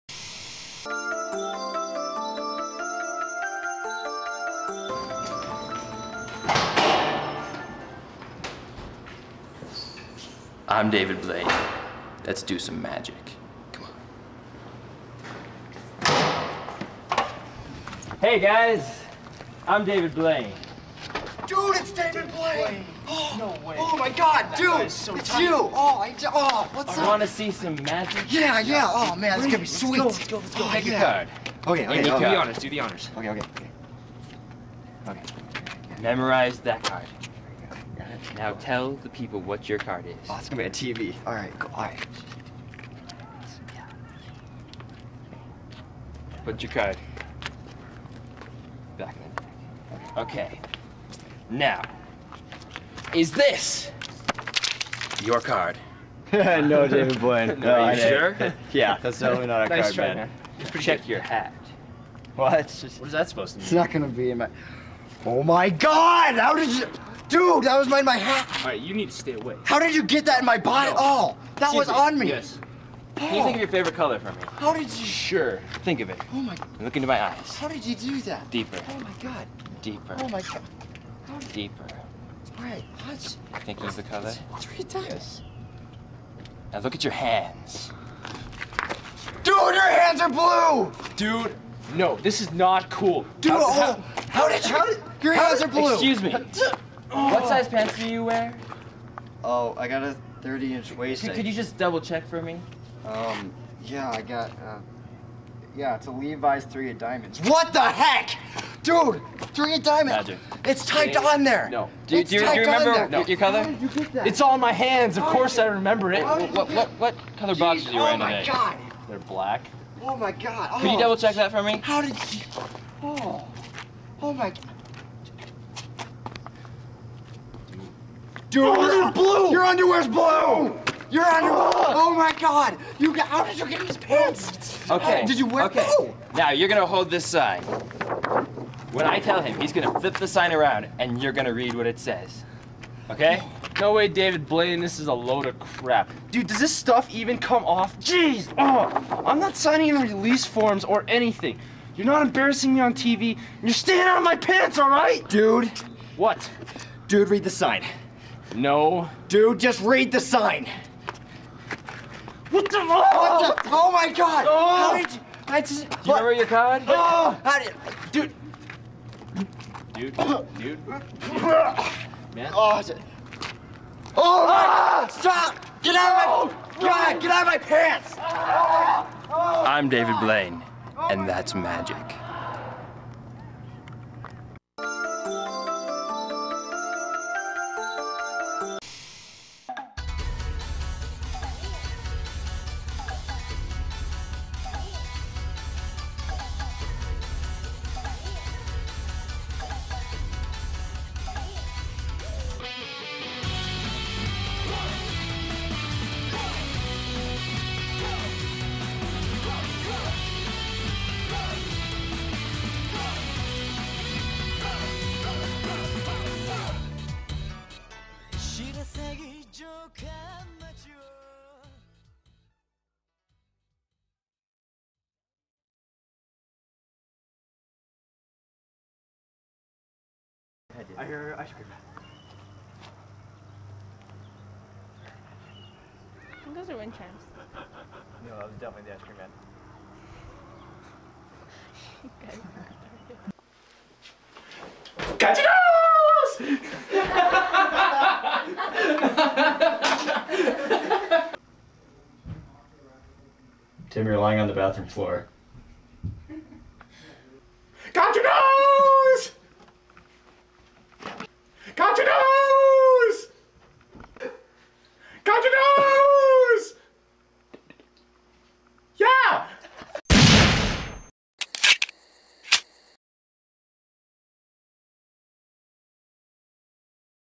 Got Your Nose! (Part 2)
2nd half of our submission to the second film festival.  It won 1st in Popular Vote and 3rd in Judge's Choice.